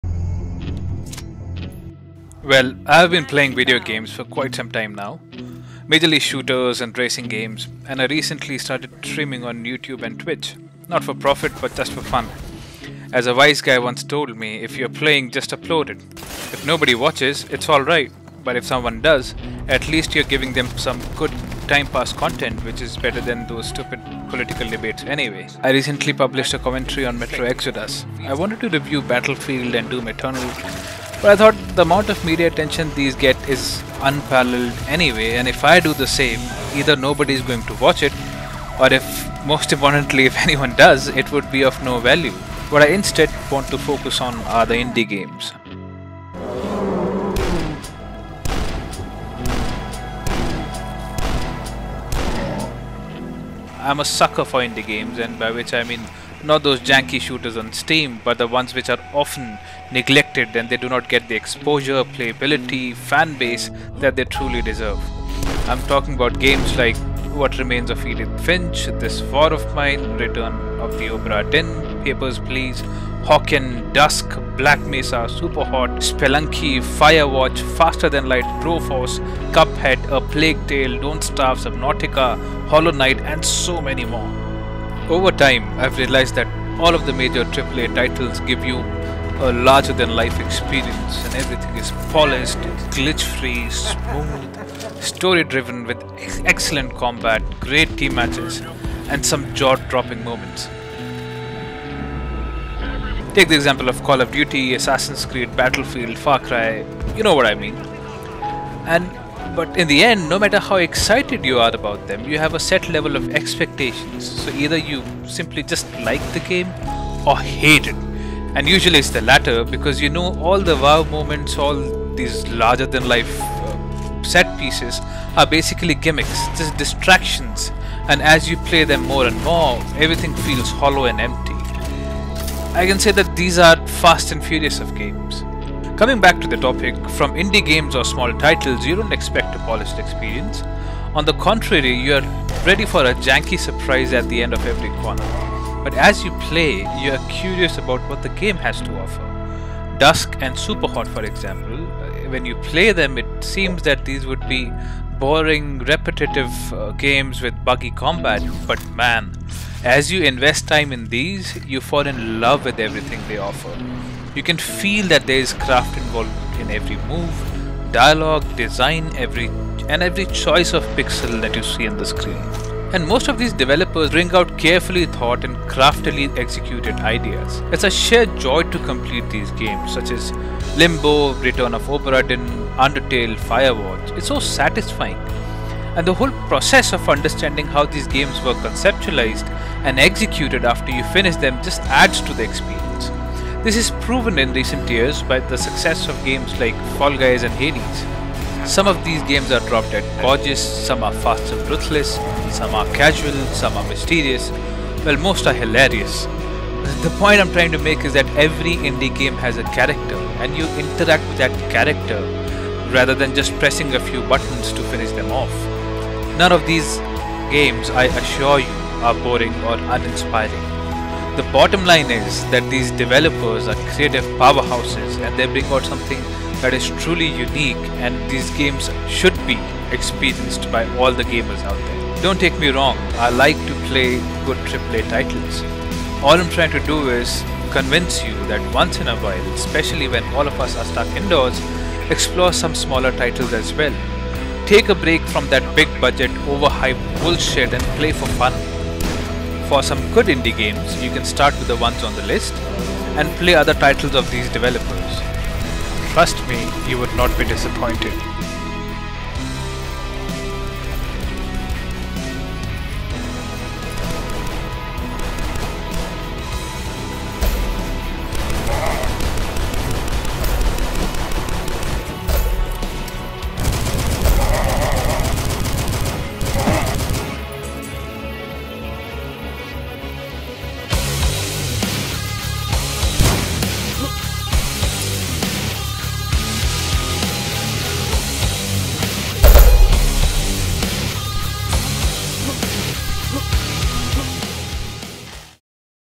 Why you should play Indie game titles
Indie games may seem boring, repetitive games with buggy combat, but as you invest time on these, you fall in love with everything. You feel the craft involved in every move, dialogue, design and every choice of the pixel on the screen.

Indie game developers are not milking their previous titles for fame and bring out carefully thought and craftily executed ideas. It's sheer joy to complete these and the process of understanding how these games were conceptualized and executed after you finish adds to the whole experience.

I love to play these unorthodox games, by which I mean games from the small, independent developers who often do not get the exposure, playability and fan base they deserve. Here is a list to consider:
• This War of Mine
• Return of the Obera Dinn
• Papers, please
• Hawken
• DUSK
• Black Mesa
• Superhot
• Journey
• Spelunky
• Firewatch
• Faster than light
• Broforce
• Cuphead
• A plague tale
• Don't starve
• Subnautica
• Hollow Night